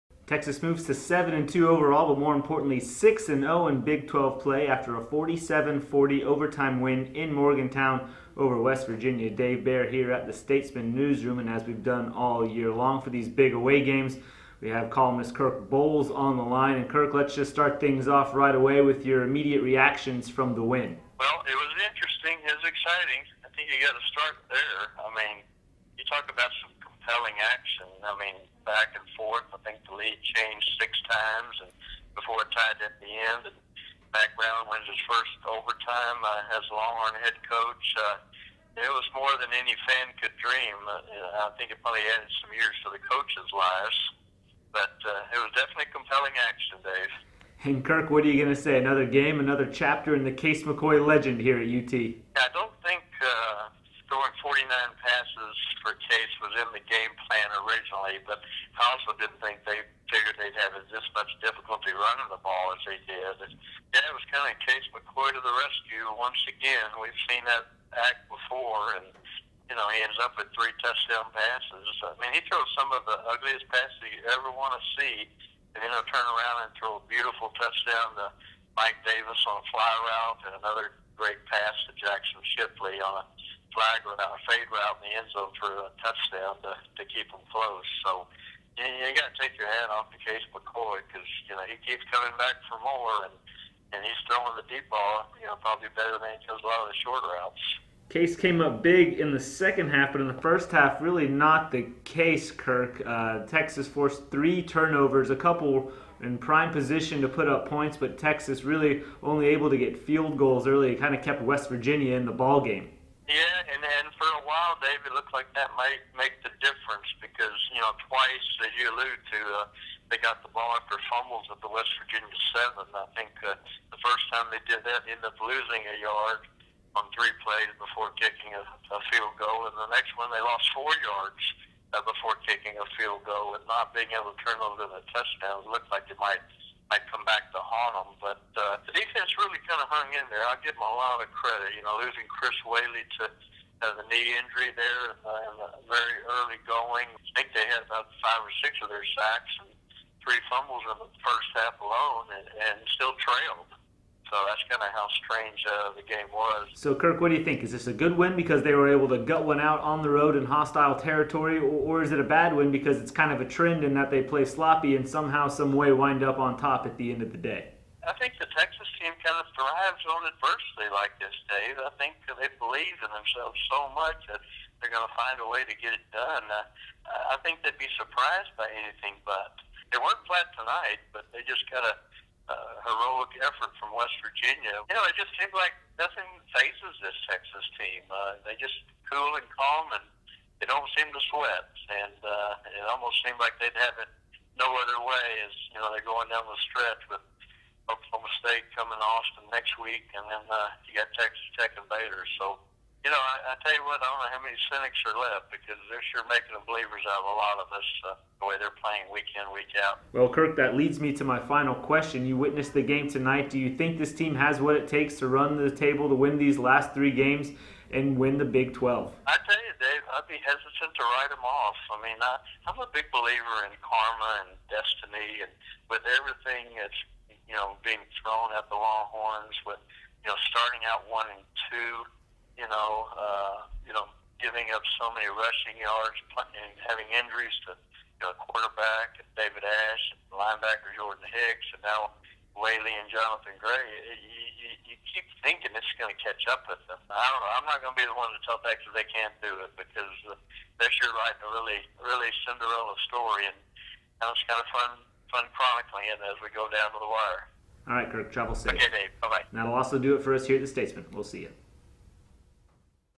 Texas escapes with win over West Virginia
Texas escapes Morgantown and stays undefeated in the Big 12 with a 47-40 overtime win over West Virginia.